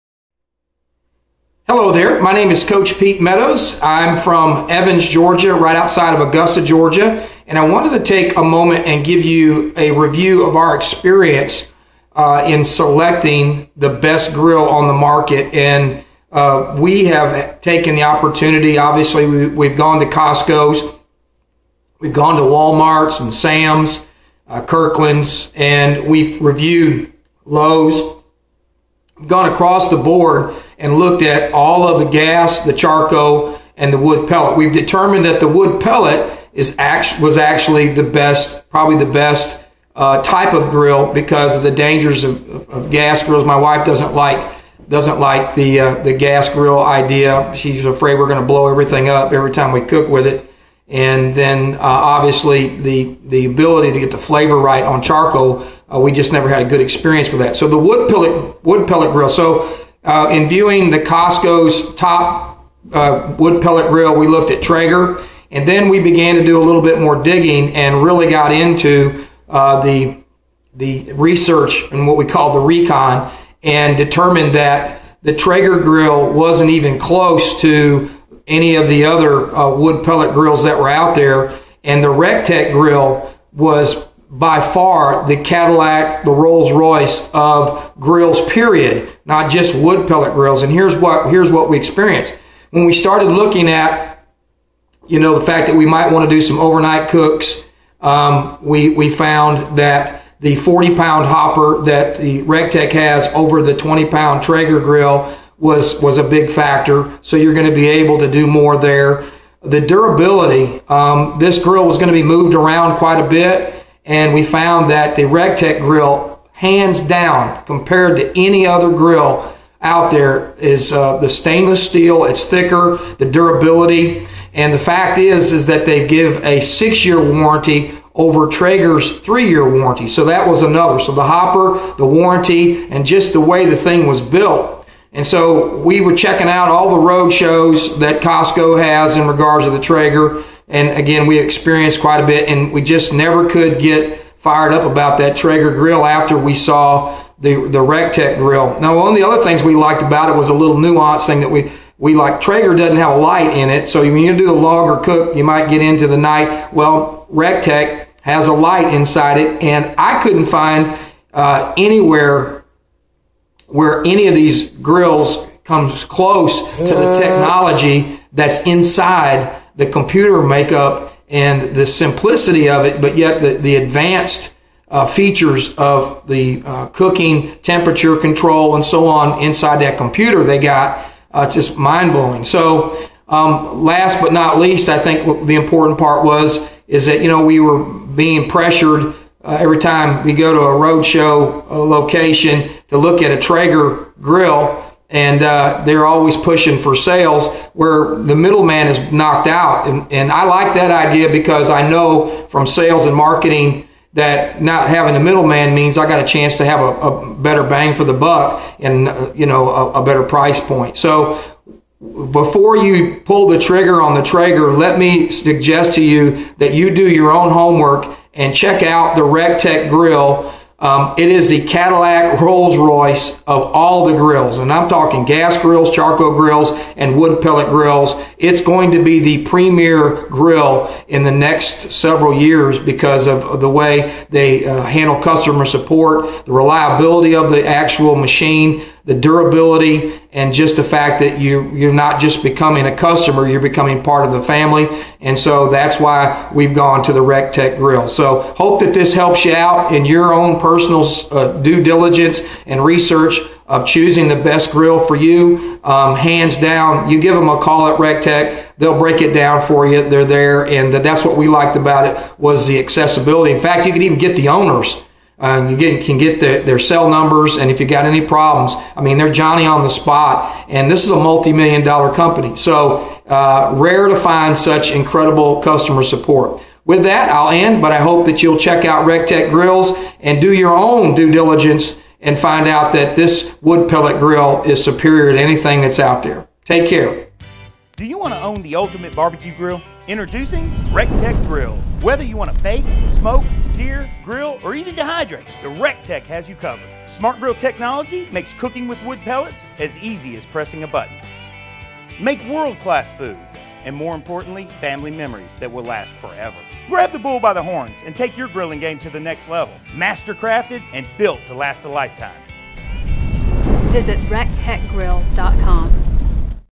Review Traeger Wood Pellet Grills vs. Rec Tec Wood Pellet Grill - Augusta, GA
This my review of the Traeger Wood Pellet Grill vs. Rec Tec Grill after reviewing all types of grills, including gas grills, charcoal grills, we settled on wood pellet style grills.

We went to Costco, Lowes, Wal Mart, and Kirkland's to view all of these grills..

After a complete review, over a two-month period, we found that the Rec Tec grill was by far the best value, with the most longevity, as well as many of the features I discuss in the video.